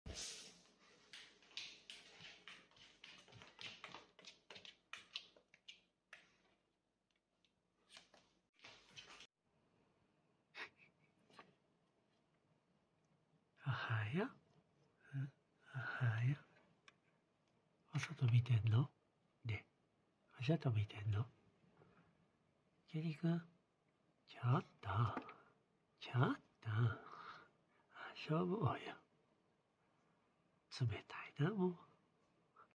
今日のジュニ君🦉
トコトコ歩きとツンデレのジュニ君🦉